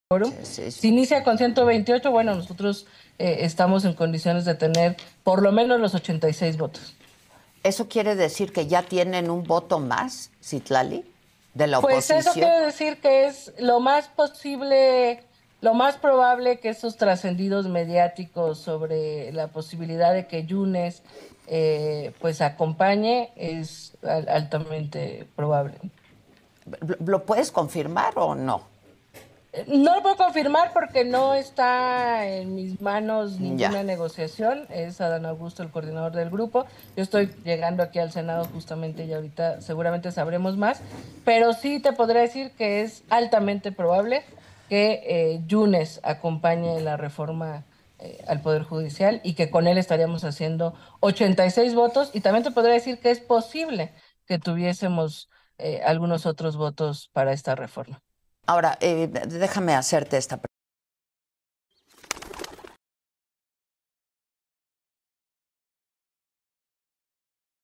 Casi es un hecho que Yunes Márquez votará a favor de la Reforma Judicial: Citlalli Hernández
Video📹 | *Casi es un hecho que Yunes Márquez votará a favor de la Reforma Judicial: Citlalli Hernández*